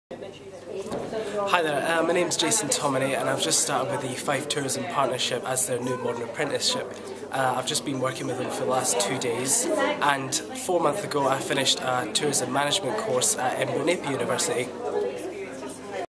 Jason Tominy, Fife Tourism Partnership
Jason Tominy of Fife Tourism Partnership at the Open Day event on 6th November 2012